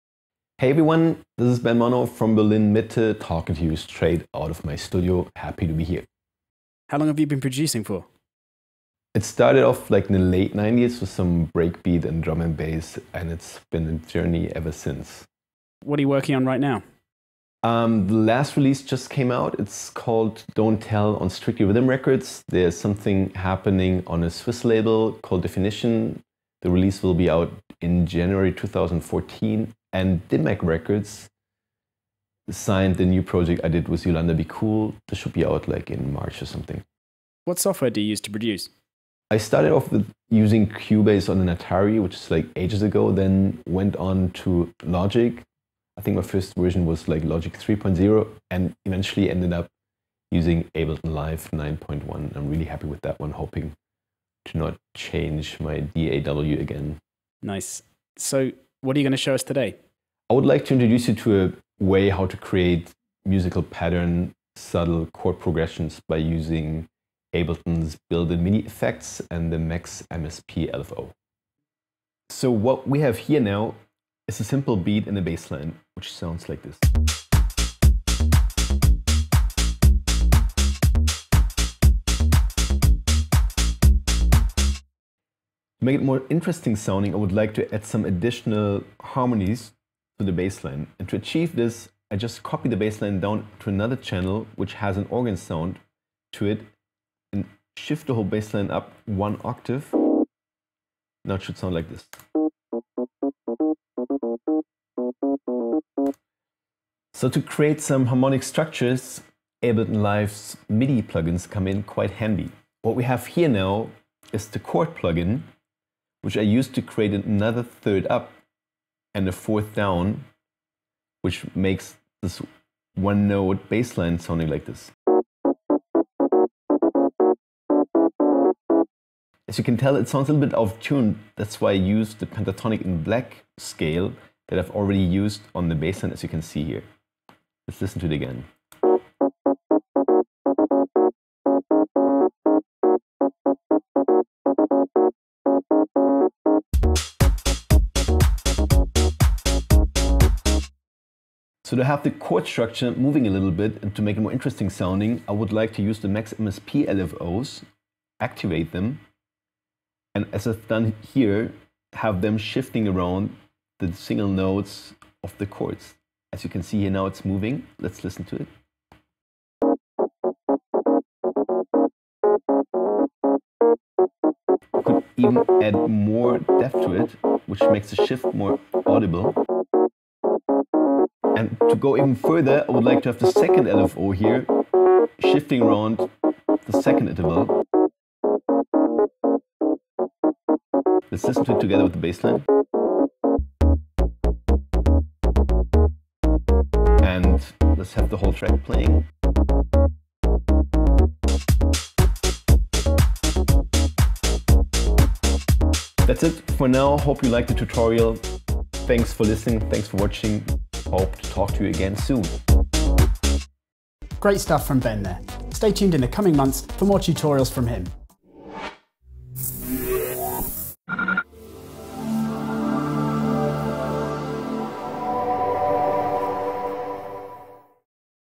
Ben Mono Interview and Ableton Live Tutorial - Excerpt from In The Loop Ep 1
This brief interview and tutorial from Berlin producer Ben Mono is an excerpt from episode 1 of our video podcast In The Loop.